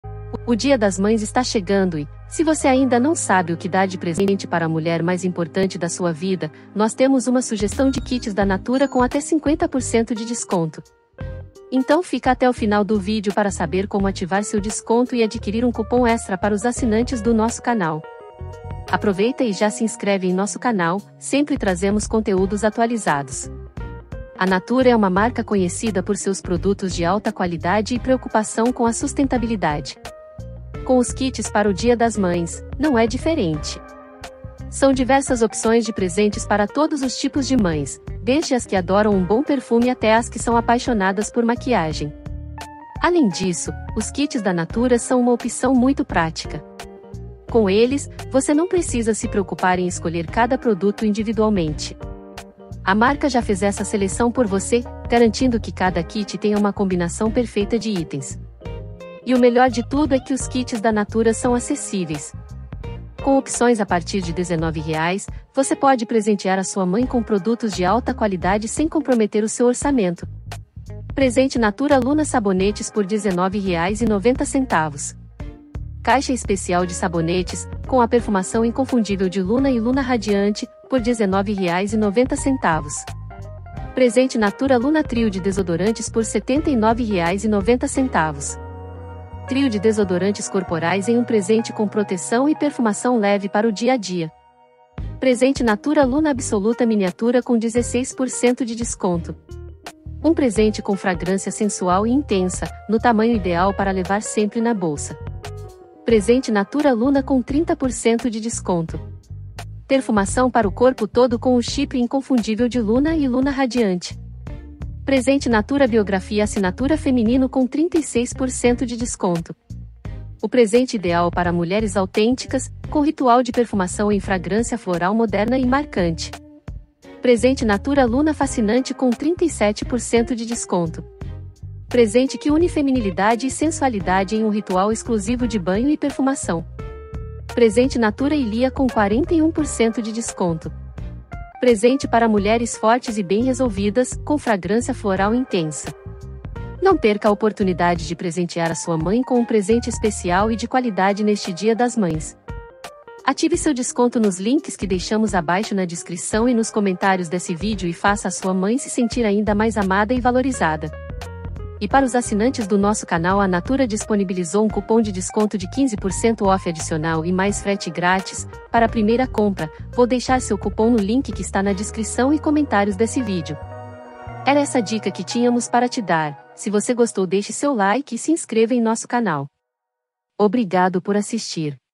Kits da Natura para presentear as Mães 2023. Kits da Natura com Desconto!
A Natura é uma marca conhecida por seus produtos de alta qualidade e preocupação com a sustentabilidade. Com os kits para o Dia das Mães, não é diferente. São diversas opções de presentes para todos os tipos de mães: desde as que adoram um bom perfume até as que são apaixonadas por maquiagem.

Além disso, os kits da Natura são uma opção muito prática. Com eles, você não precisa se preocupar em escolher cada produto individualmente. A marca já fez essa seleção por você, garantindo que cada kit tenha uma combinação perfeita de itens.

00:05 O que dá de presente no dia das mães?
00:12 Kits da Natura com desconto
00:30 Presentes de qualidade para as mães
00:32 Kits para o dia das mães
00:38 Opções de presentes para os diversos tipos de mães
00:40  Presentes para as mães que adoram perfume
00:42 Presente para as mães que gostam de maquiagem
01:06 Kits da Natura bons e baratos para presentear as mães
01:17 Kit Presente Natura Luna Sabonetes
01:24 Kit Presente Natura Luna Trio de Desodorantes
01:35 Kit Presente Natura Luna Absoluta Miniatura
01:46 Kit Presente Natura Luna
01:55 Kit Presente Natura Biografia Assinatura Feminino
02:08 Kit Presente Natura Luna Fascinante
02:18 Kit Presente Natura Ilía